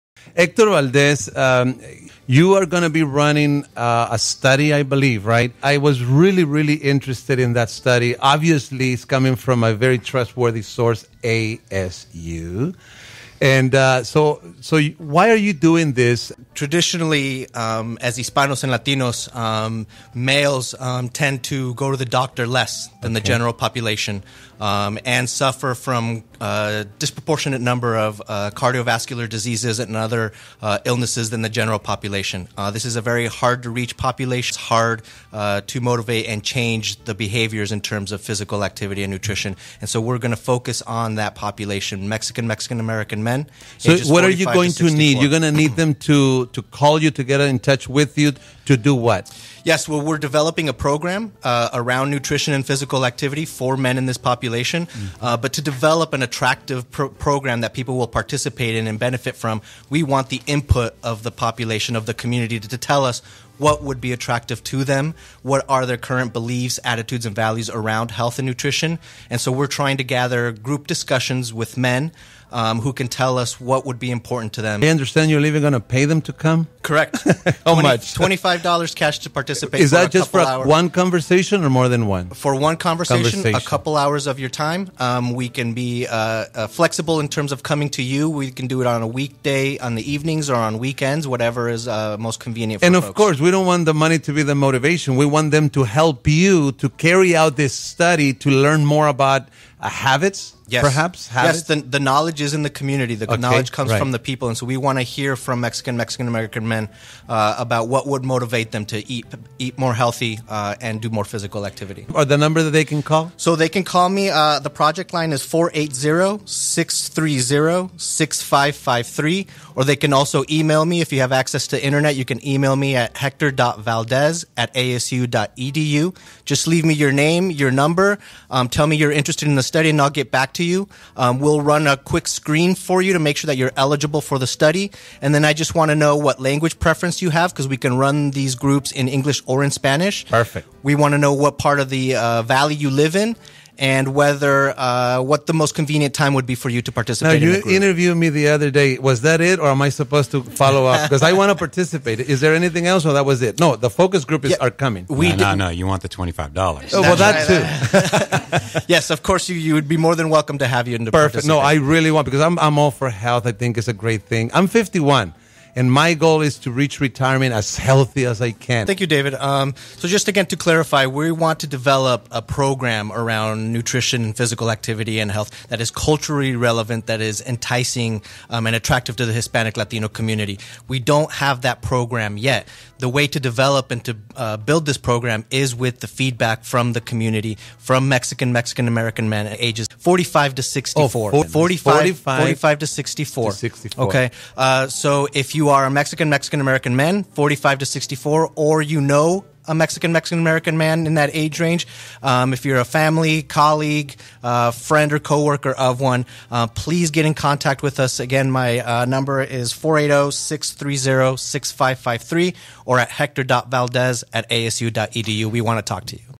ASU Health Study: diet and exercise
ASU is conducting a health study for Mexican, Mexican-American 45-64 year-old males.